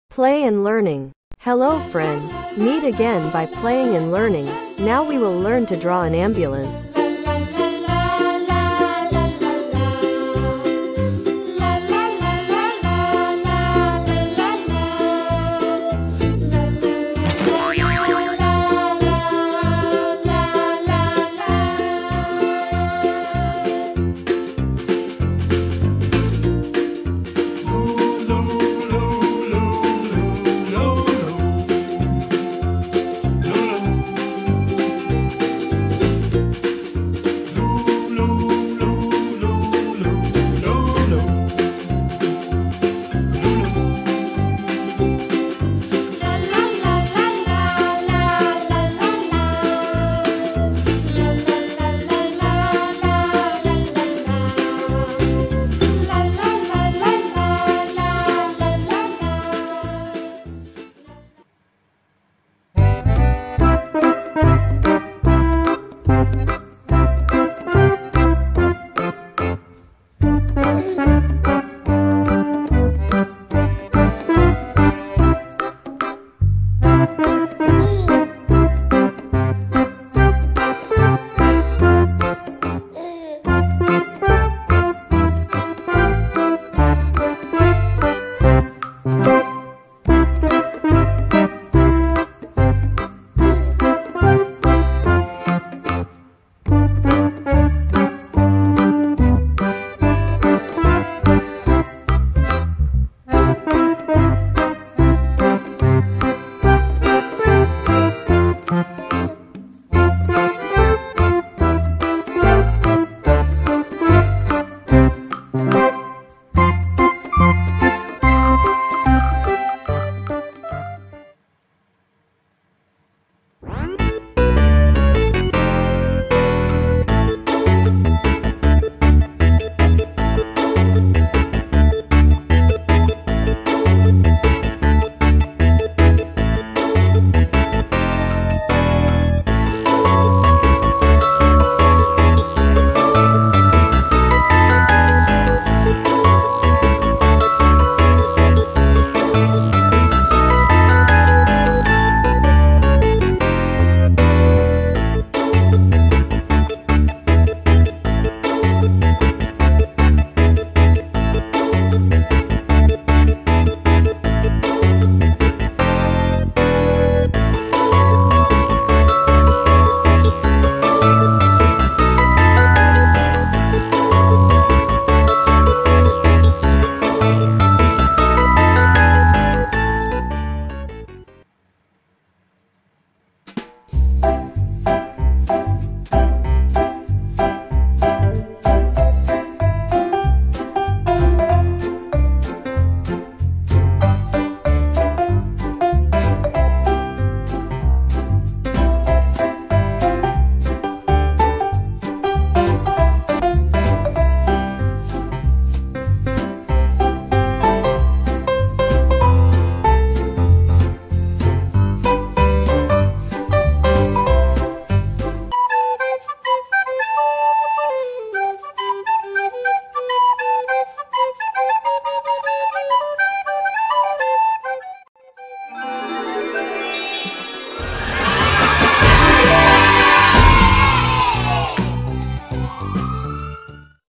Drawing and coloring a fire truck
Hi friends, have you ever seen a fire engine, this time we will learn to draw and color a fire engine, let's join in the fun